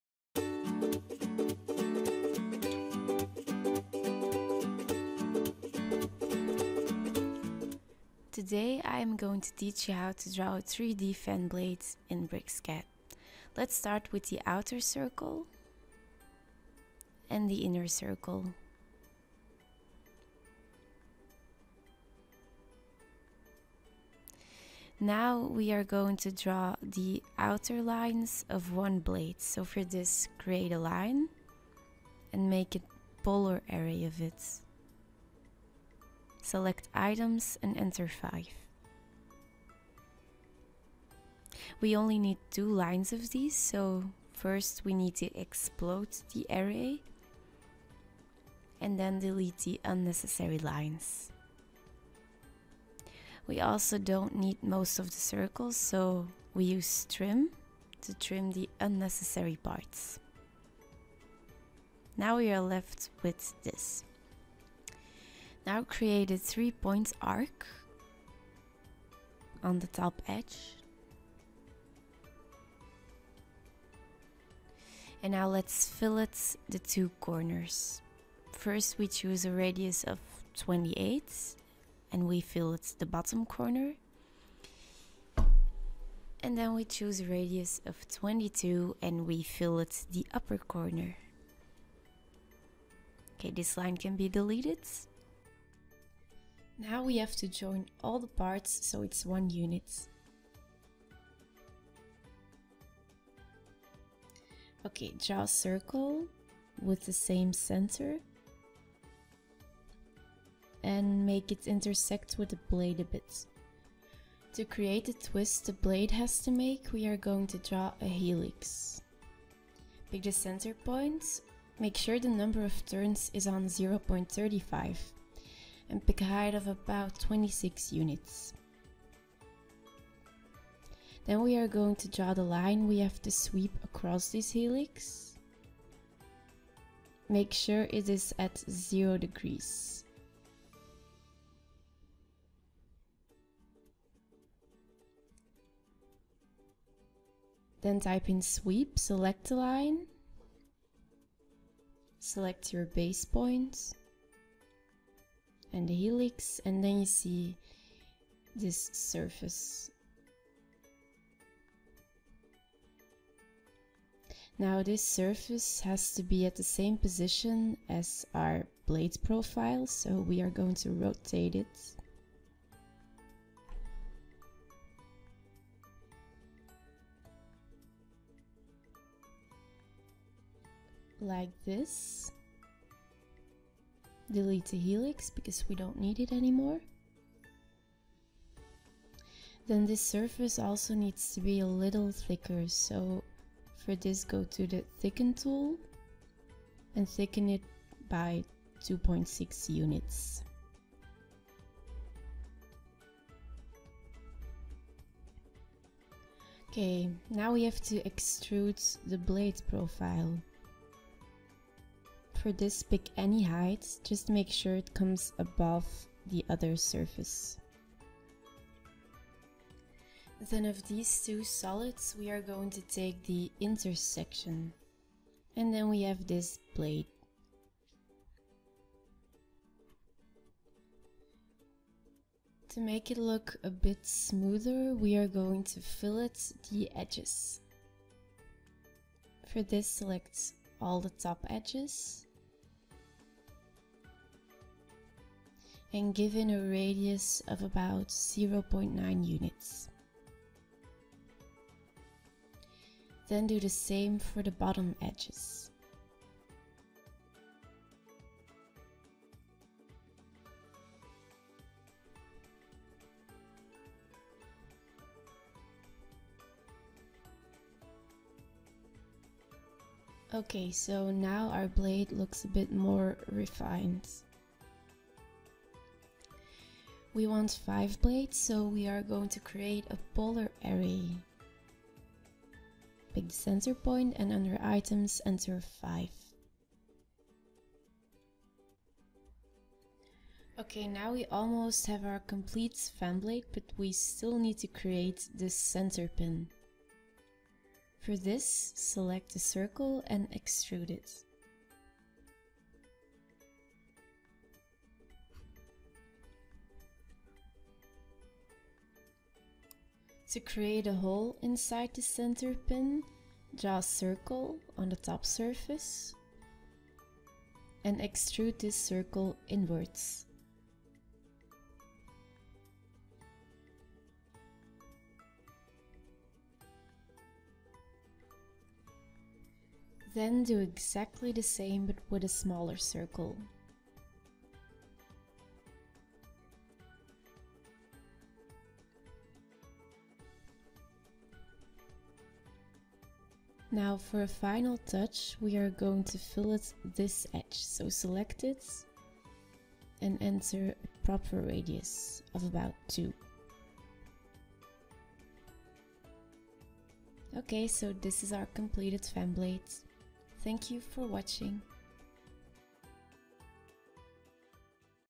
How to Draw a 3D Fan Blade in BricsCAD
In this video you will learn how to draw a 3D fan blade in BricsCAD.